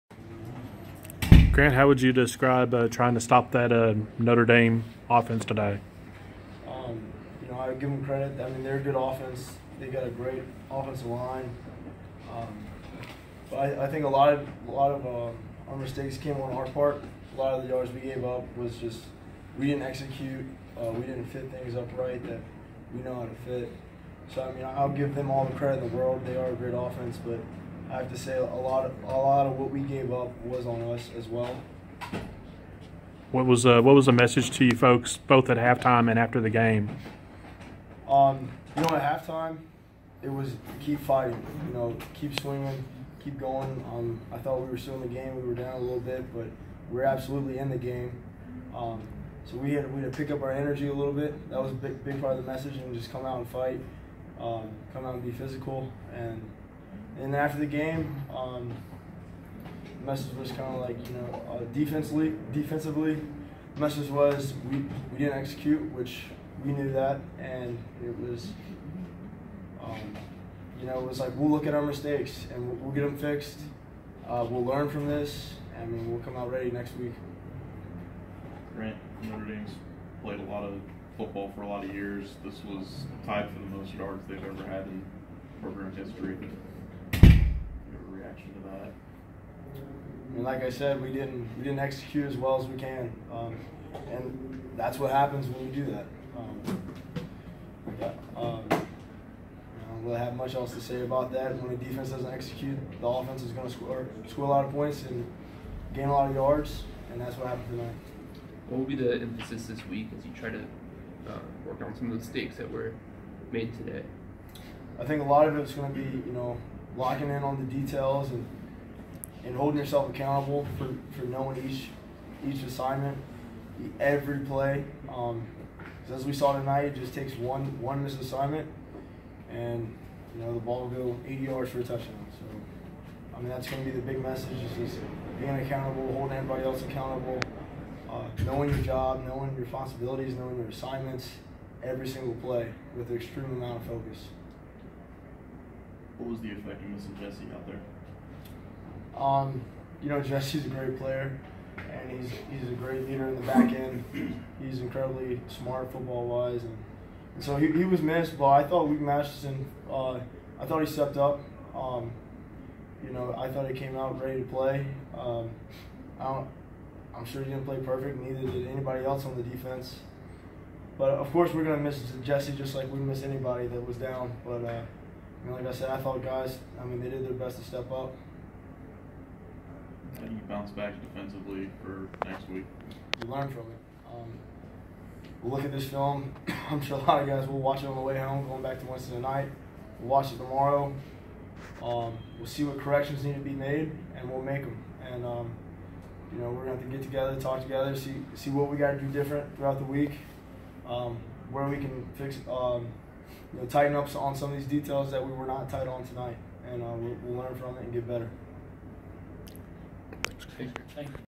Grant Dawson post Notre Dame
Wake Forest LB Grant Dawson on the Deac 48-37 loss at Notre Dame